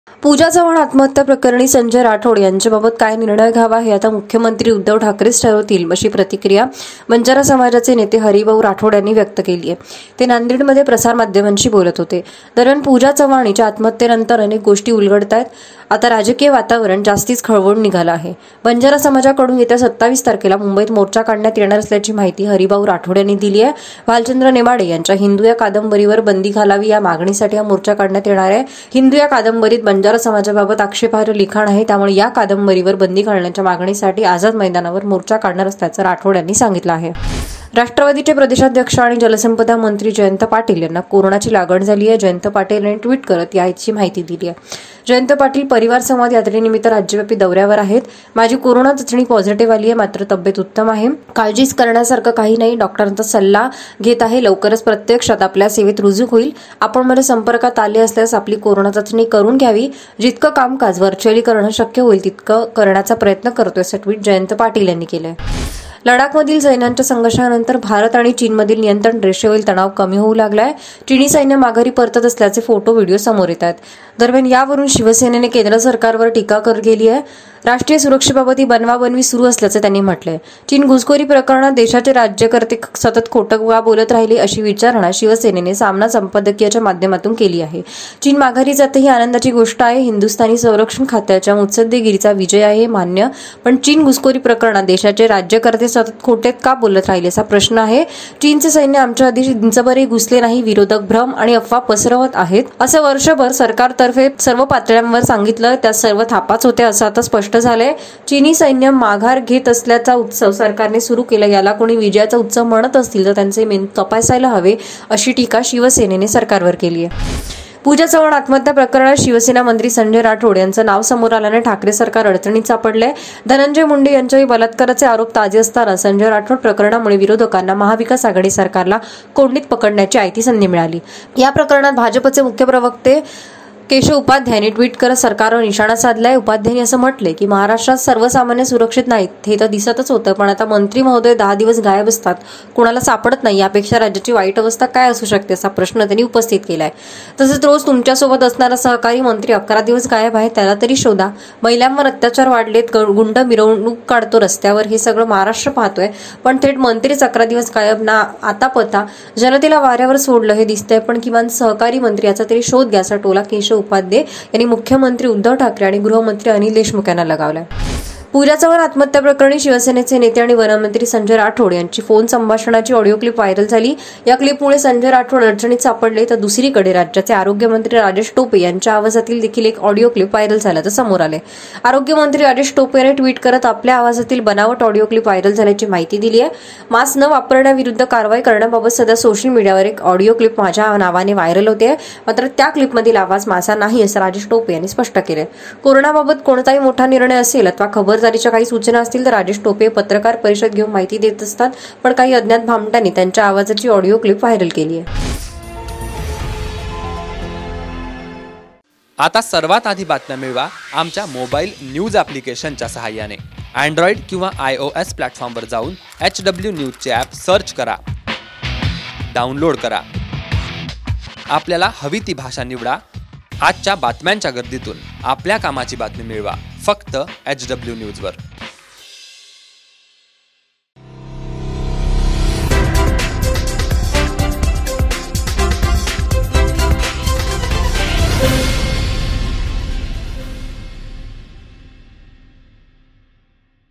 ‘महाराष्ट्राचे मंत्री ११ दिवस गायब’ जनता वाऱ्यावर आहेचं पण मंत्र्यांना तरी शोधा !
पूजा चव्हाण आत्महत्या प्रकरणात शिवसेना मंत्री संजय राठोड यांचे नाव समोर आल्याने ठाकरे सरकार अडचणीत सापडलं आहे. धनंजय मुंडे यांच्यावरील बलात्काराचे आरोप ताजे असतानाच संजय राठोड  प्रकरणामुळे विरोधकांना महाविकास आघाडी सरकारला कोंडीत पकडण्याची आयटी संधी मिळाली आहे. प्रकरणात भाजपाचे मुख्य प्रवक्ते यांनी ट्विट करून सरकारवर निशाणा साधला आहे. उपाध्ये म्हणाले की, महाराष्ट्रात सर्वसामान्य सुरक्षित नाहीत हे तर दिसतच होत पण आता मंत्री महोदय १० दिवस गायब असतात कुणालाच सापडत नाही यापेक्षा राज्याची वाईट अवस्था काय असू शकते? असा प्रश्न त्यांनी उपस्थित केला आहे. तसेच रोज तुमच्या सोबत बसणारा सहकारी मंत्री ११ दिवस गायब आहे त्याला तरी शोधा, महिलांवर अत्याचार वाढलेत, गुंड मिरवणूक काढतो रस्त्यावर हे सगळ महाराष्ट्र पाहतोय पण थेट मंत्रीच ११ दिवस गायब. ना आता ना पता. जनतेला वाऱ्यावर सोडल हे दिसतय पण किमान सहकारी मंत्री त्याचा तरी शोध घ्या असा टोला केशव उपाध्ये यांनी मुख्यमंत्री उद्धव ठाकरे आणि गृहमंत्री अनिल देशमुख यांना लगावला आहे.